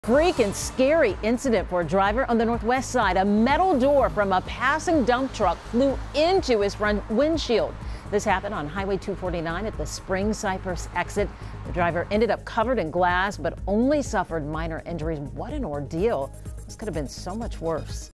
Metal door crashes right through windshield of car
The driver was injured but those injuries aren't believed to be life-threatening.